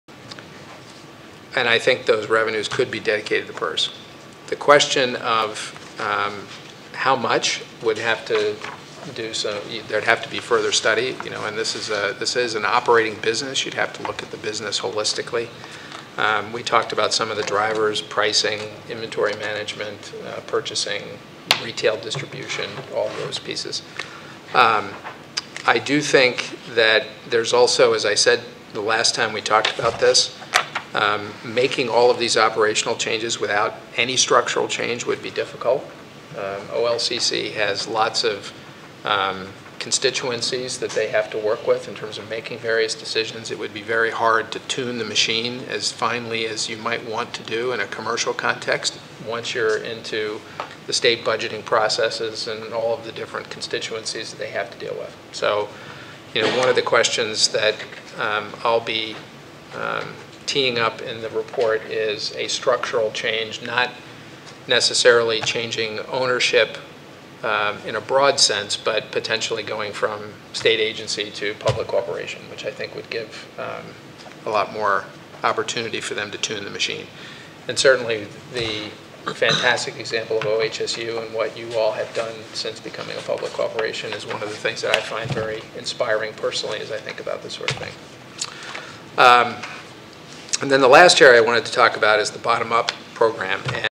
Better OLCC could mean better PERS
Don Blair, chair of Gov. Kate Brown's PERS task force, says the Oregon Liquor Control Commission could improve its operations and structure, with the resulting increased income then devoted to reducing PERS' $25.3 billion unfunded actuarial liability. The Oregon Public Employees Retirement System task force is trying to come up with $5 billion to reduce that liability. The task force held its fourth and final meeting Oct. 13 and will deliver its report to Brown on Nov. 1.